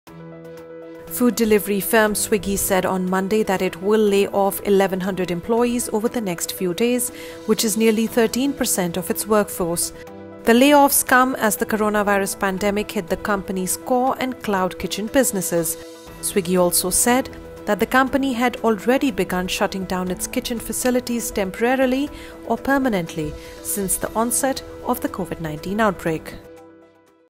Swiggy to layoff 1,100 employees as Covid-19 hits business
Food delivery firm Swiggy said on Monday that it will lay off 1,100 employees over the next few days, which is nearly 13% of its workforce. The lay offs come as the coronavirus pandemic hit the company's core and cloud kitchen businesses. Swiggy also said the company had already begun shutting down its kitchen facilities temporarily or permanently since the onset of the Covid-19 outbreak.